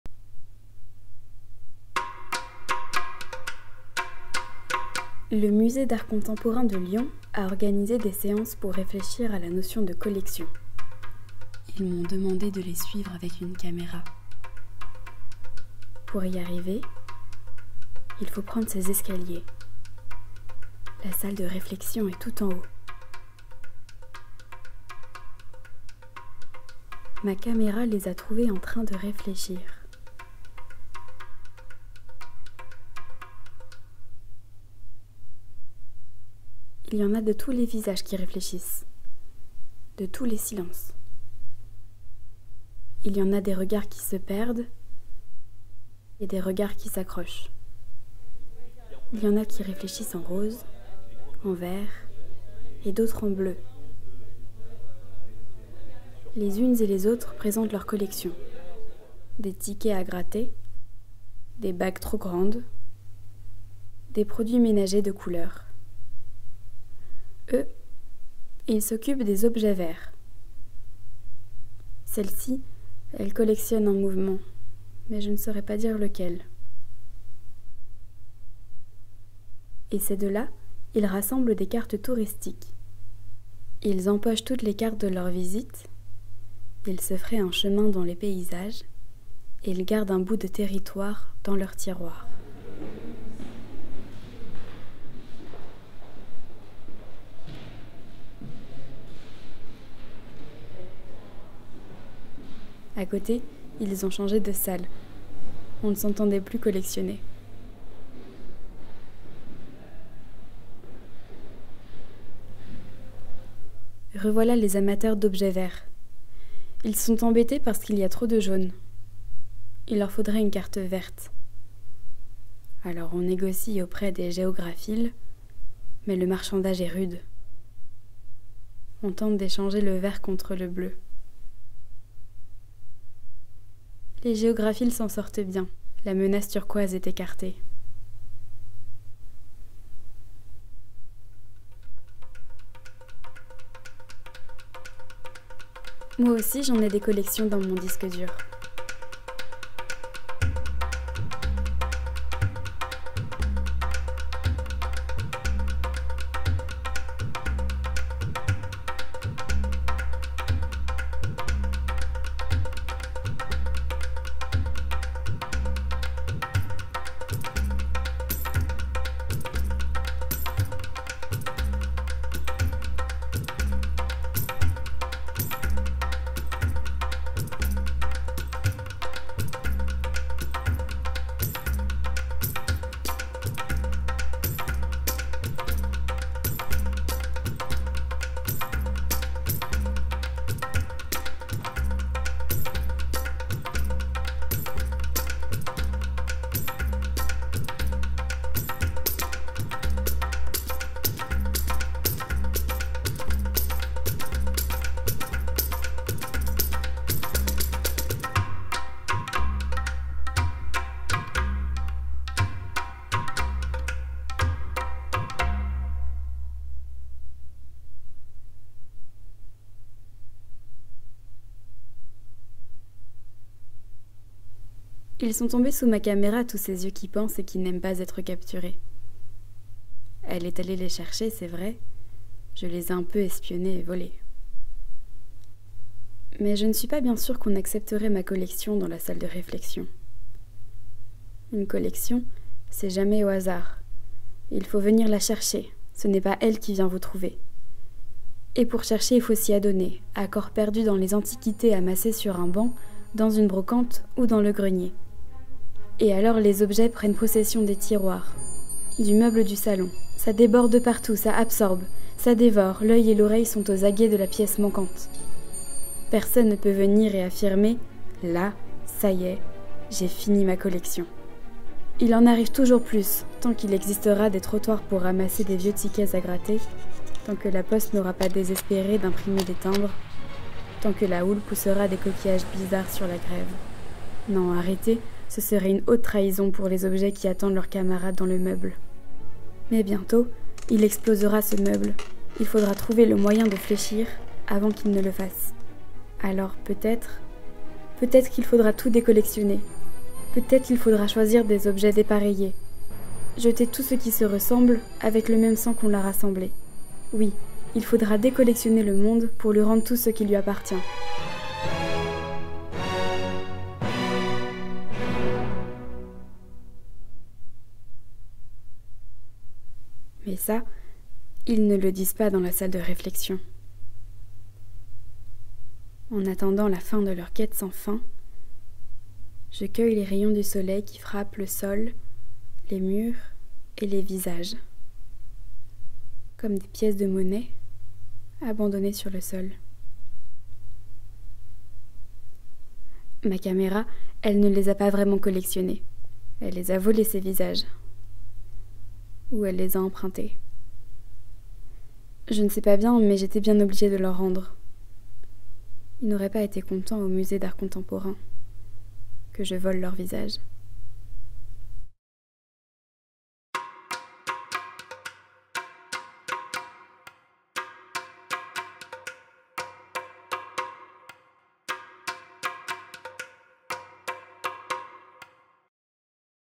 Décollection - macSUP #6
Pour macSUP #6, Salomé Kerrand filme les coulisses et apporte son regard sur ce programme de recherche-création en milieu universitaire.

Les groupes proposeront ensuite des journées expérimentales au musée : ateliers, forums, parcours participatifs… pour faire vivre aux visiteur·euses du musée ce qui a constitué leur expérience tout au long de macSUP.
Rendez-vous au musée les 1er et 2 avril 2023 au macLYON !